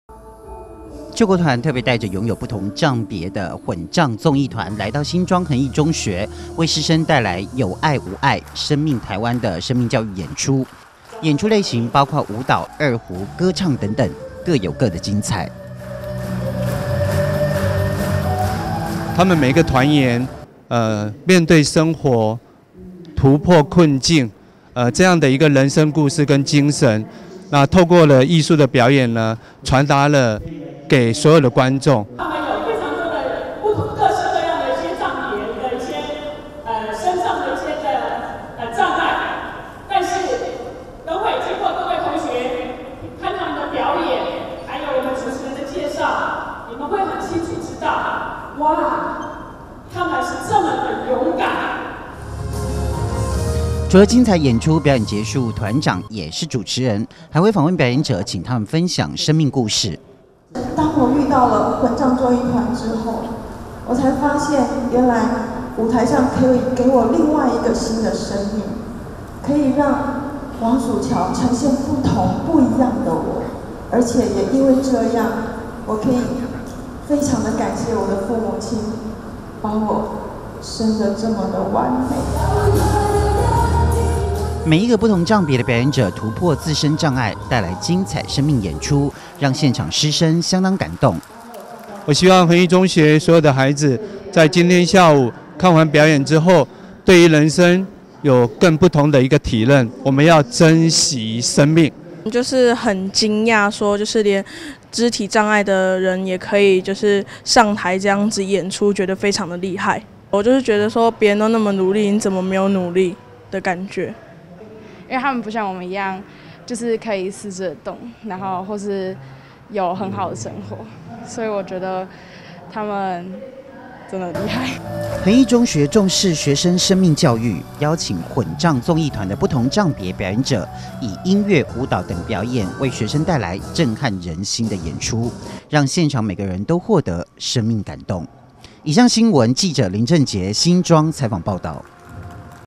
恆毅中學邀混障綜藝團演出 感動師生
救國團特別帶著擁有不同障別的混障綜藝團來到新莊恆毅中學為師生帶來有愛無礙 生命台灣的生命教育演出，演出類型包括舞蹈 二胡 歌唱等等，表演者結束還會分享自己的生命故事，加上他們勵志精彩的演出，讓所有師生獲得生命感動。